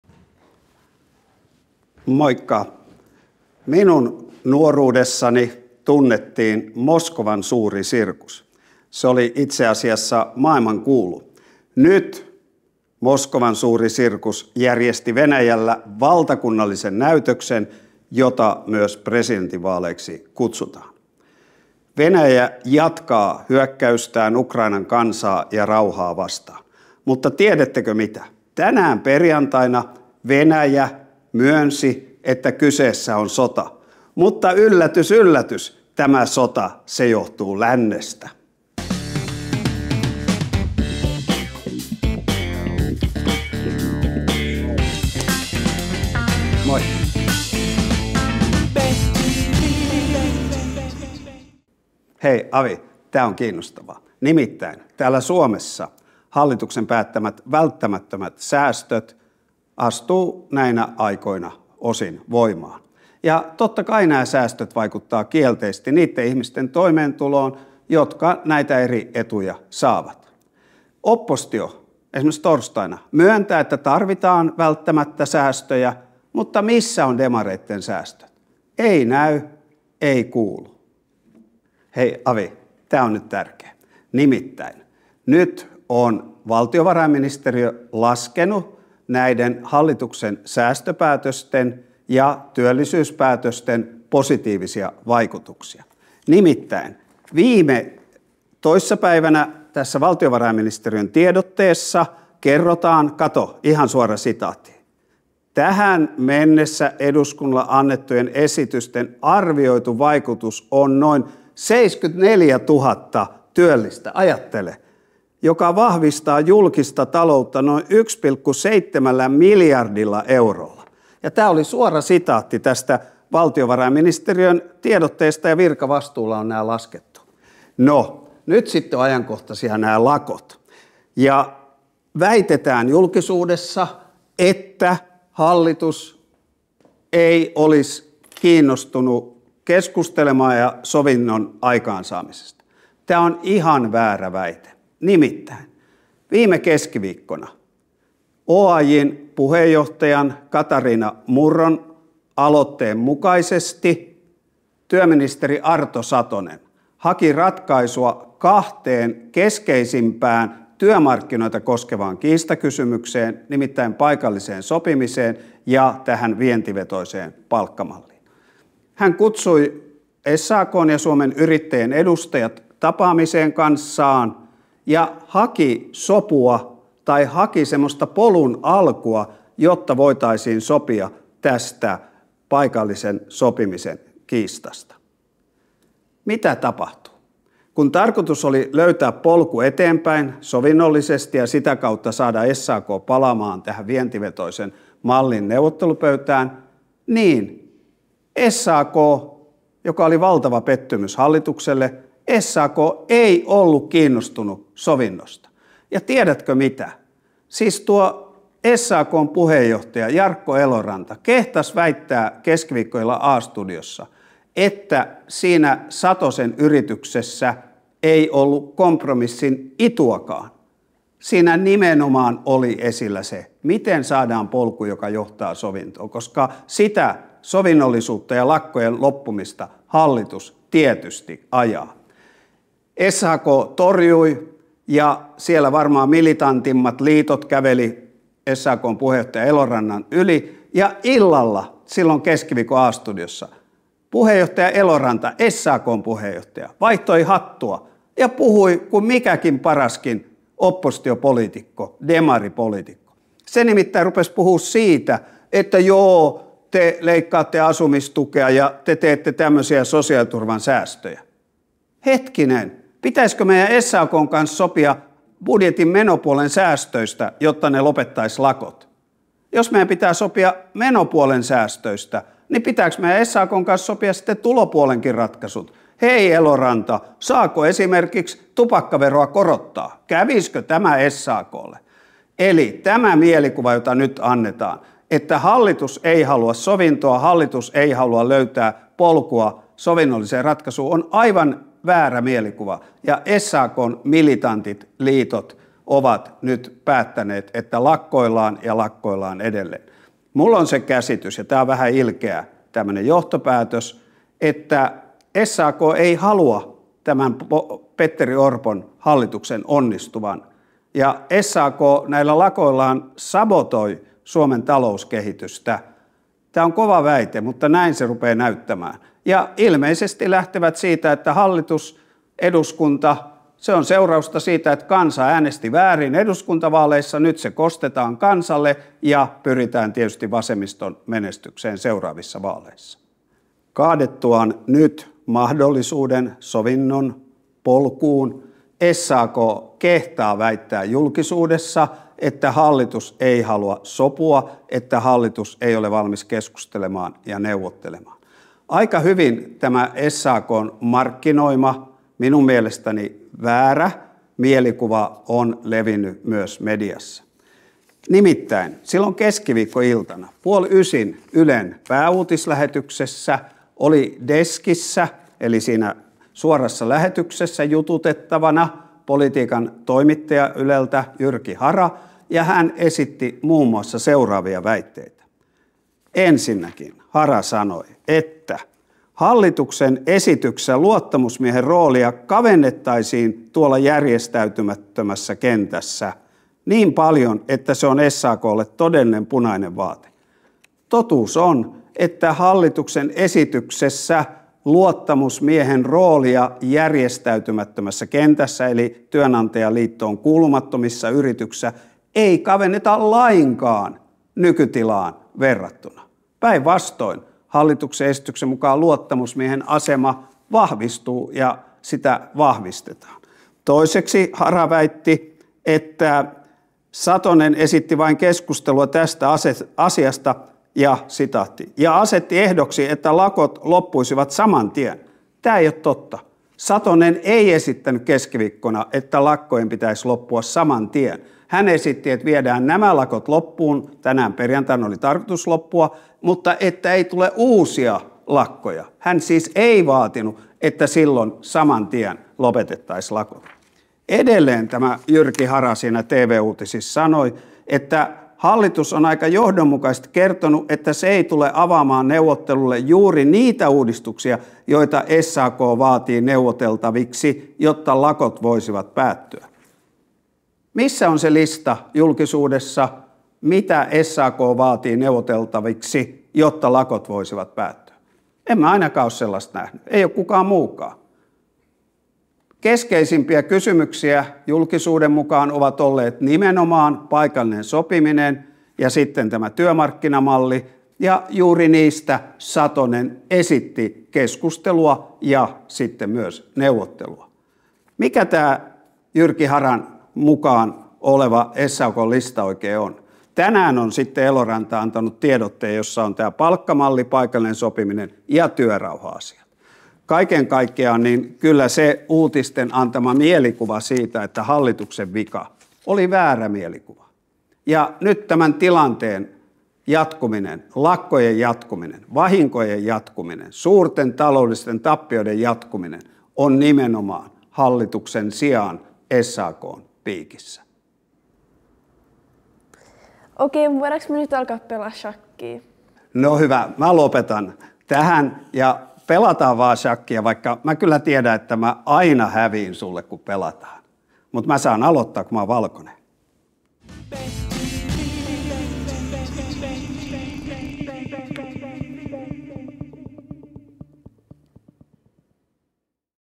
BenTV: SAK sabotoi lakoillaan Suomen talouskehitystä
Ammattiliitto ei halua Petteri Orpon hallituksen onnistuvan, arvio Ben Zyskowicz.